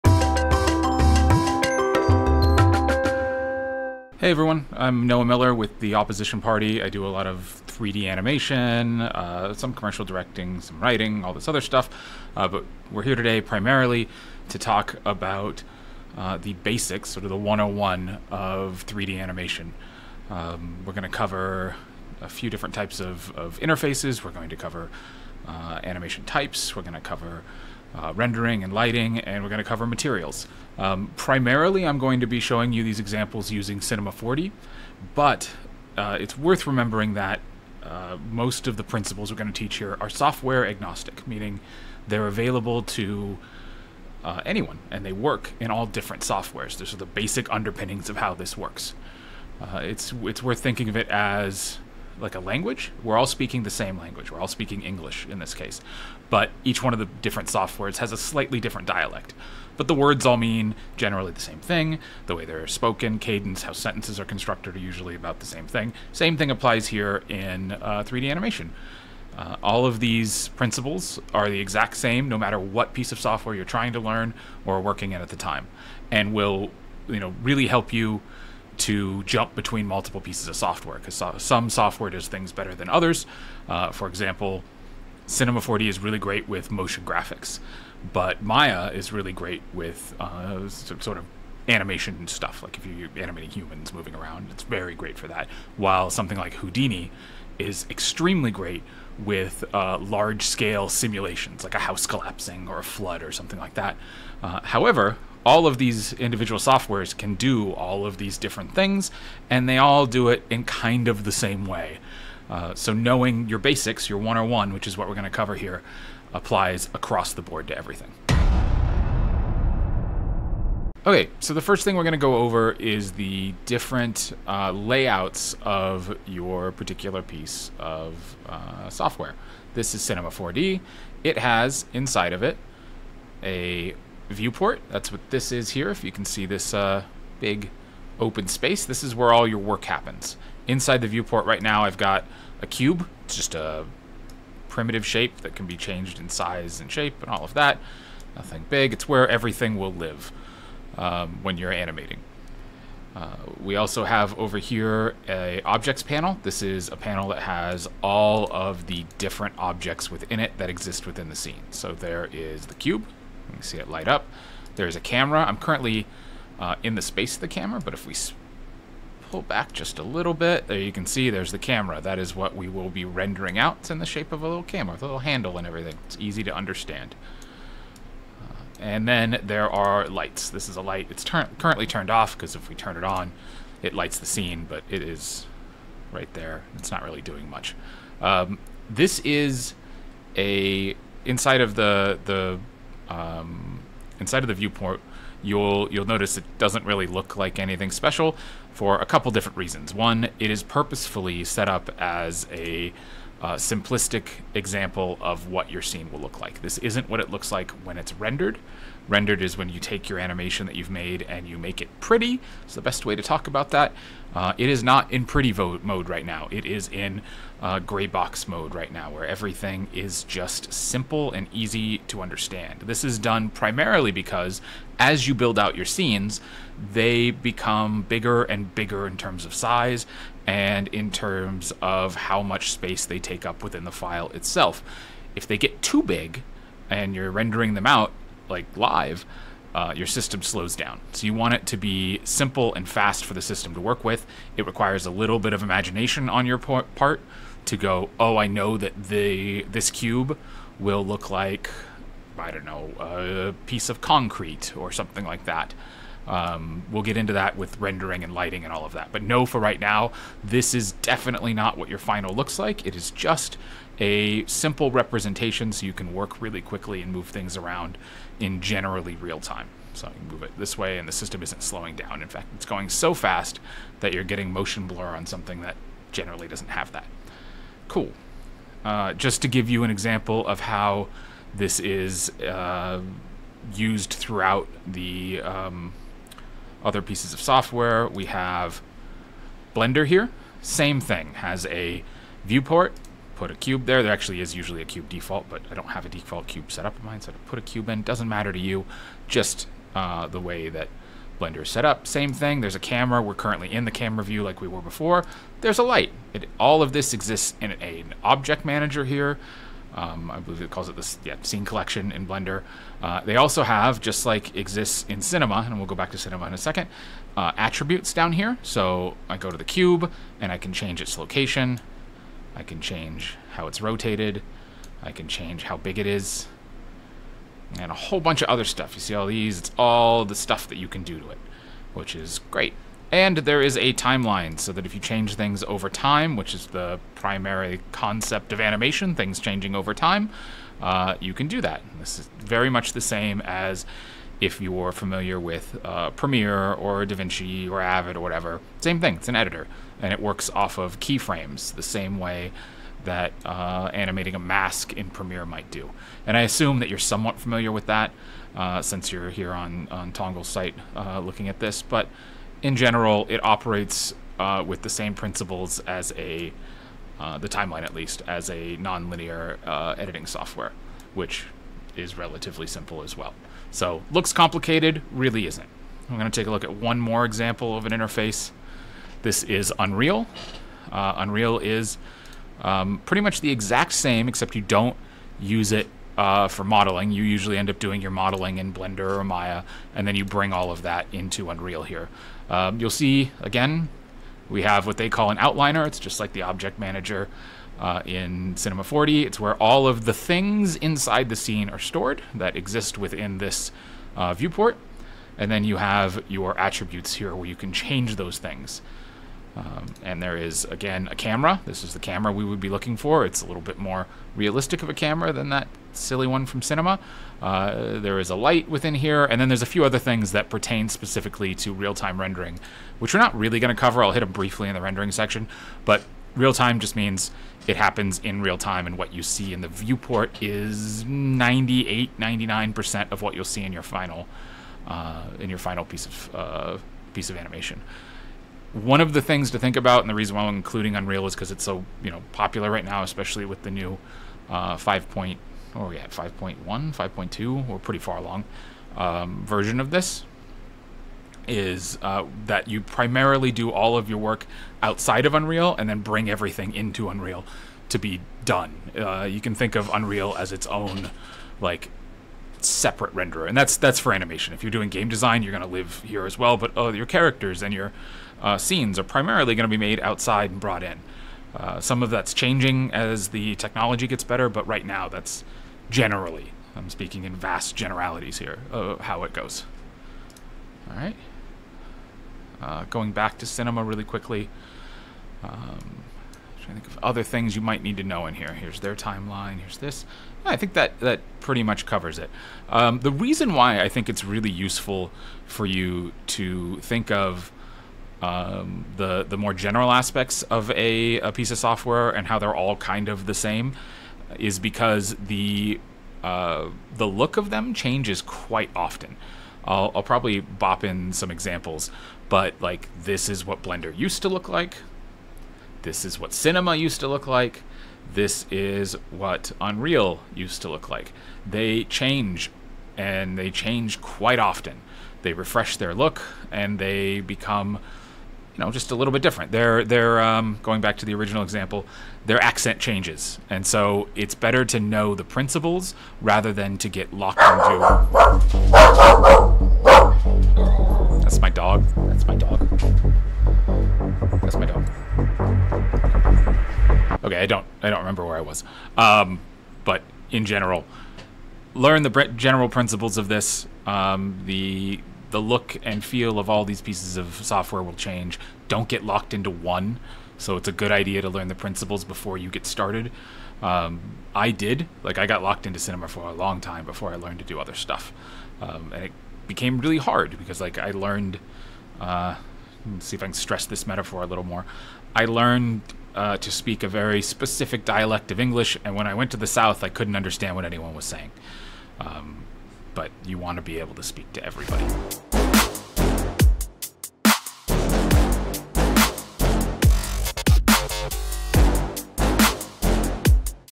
Unreal Cinema4D Blender Animation Tutorial - Lesson 1: Interfaces
All CG programs generally have the same interface that you use to interact 1 with your 3d objects. In this video, we’ll examine 3 different programs, Unreal, Cinema4d and Blender, comparing the interfaces and their intended uses.

This video was created by Tongaler Noah Miller

About Tongal: Tongal is the ultimate platform for content creation, used by studios, brands and talent all over the world.